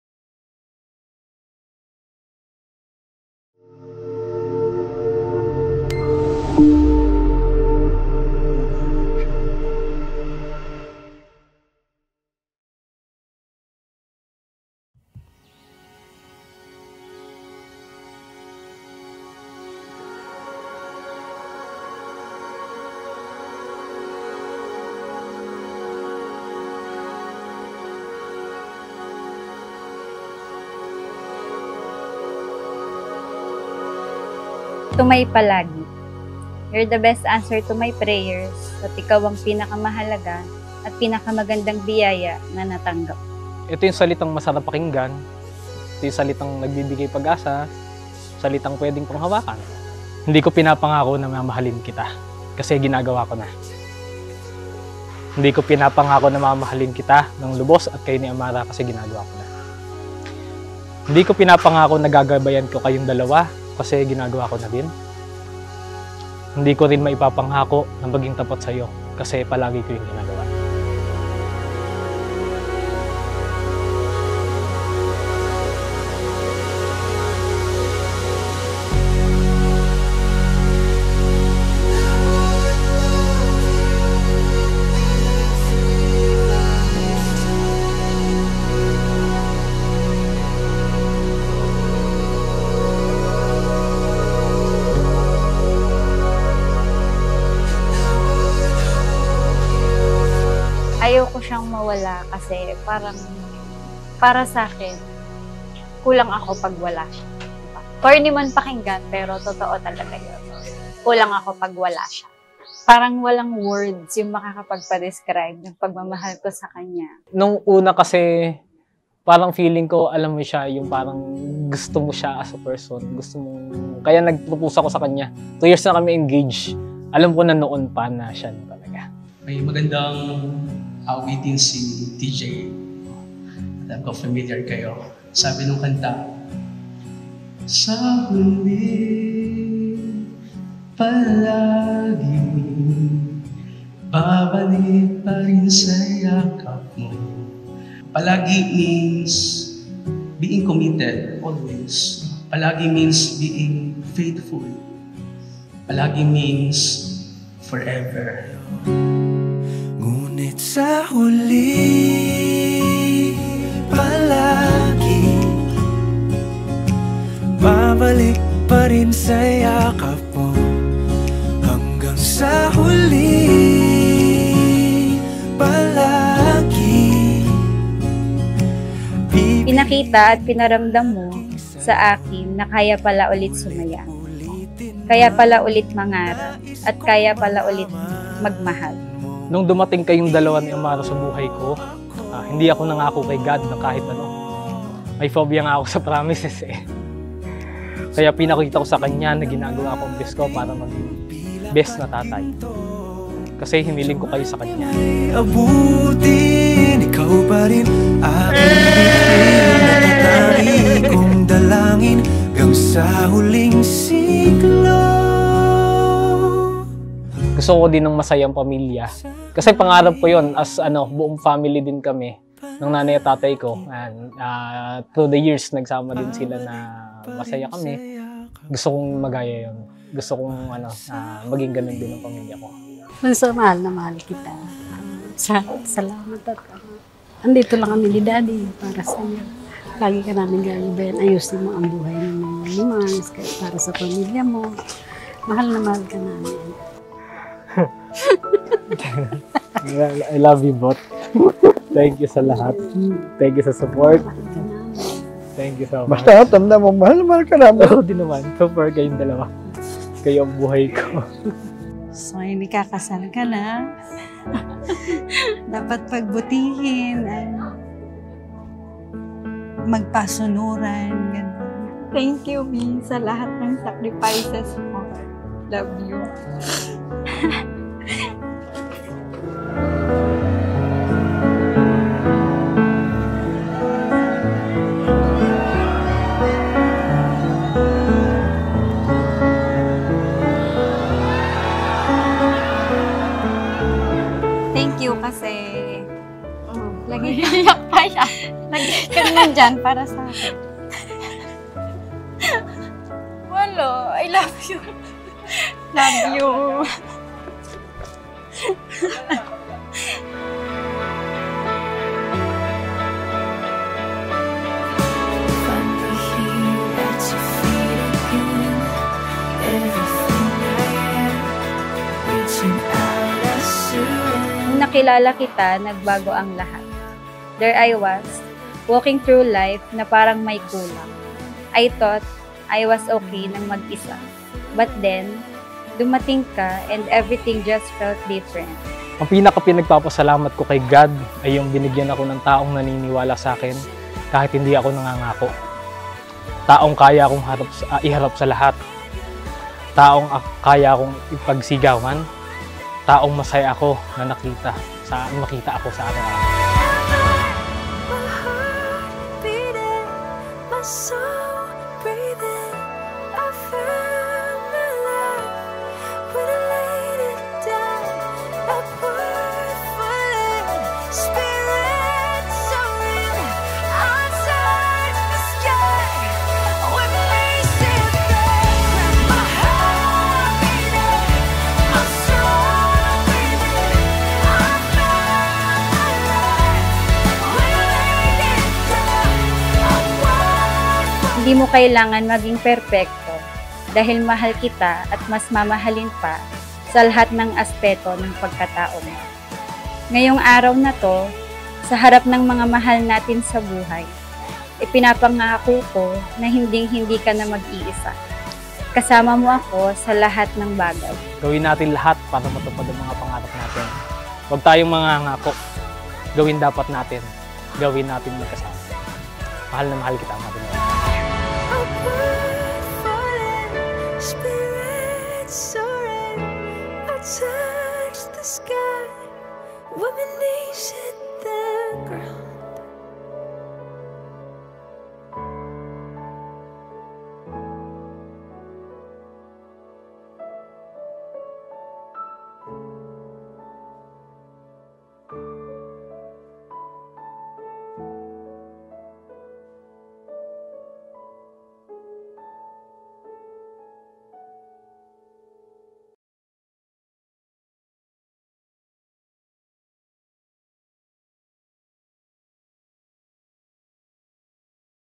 WEDDING - LEO & MARINELA | SDE
Leo & Marinela Wedding | Same Day Edit Video

Preparation: Casa Roma Mamatid Cabuyao Laguna
Ceremony: Diocesan Shrine and Parish of San Vicente Ferrer - Mamatid, Cabuyao City, Laguna
Reception: Casa Roma Mamatid Cabuyao Laguna